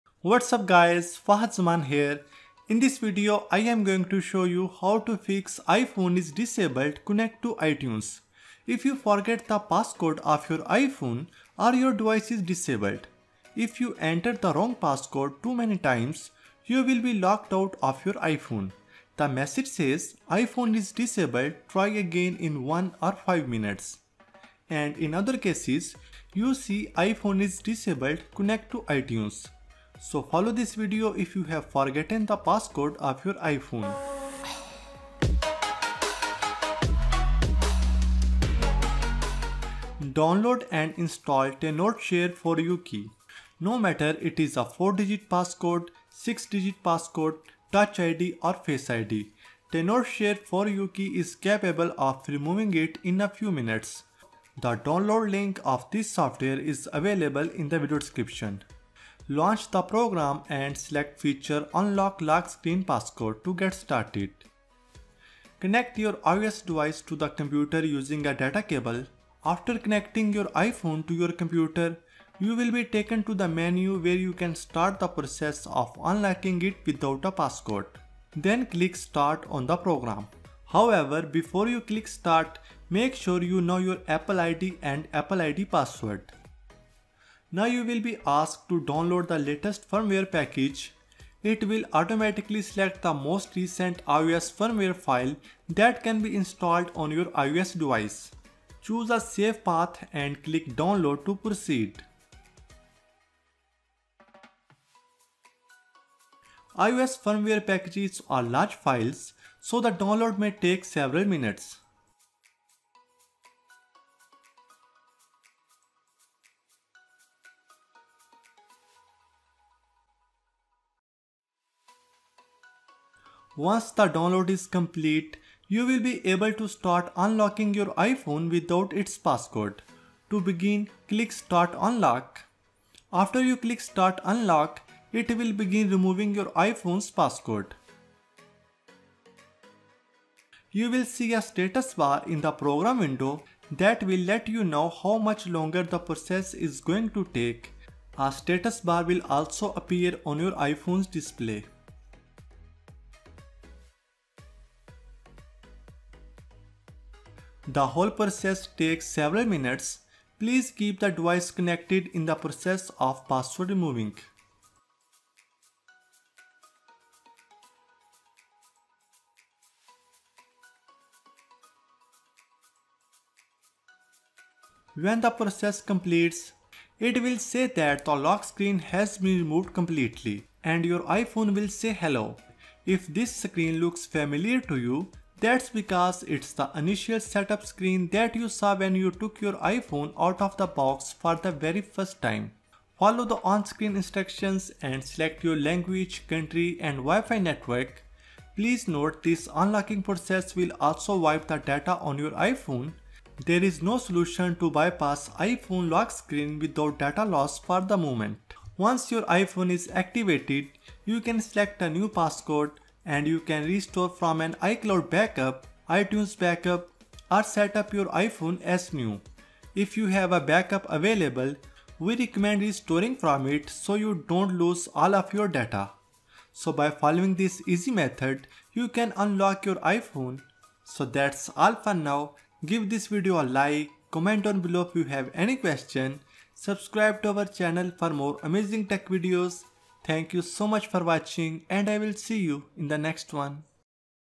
iPhone is disabled connect to iTunes| Reset iPhone Passcode 2020
How to fix iphone is disabled connect to itunes. Unlock disabled iphone 6, 7, 8, X, XS, XR, 11 Pro.

iOS 14 vs Android: Copy or Not?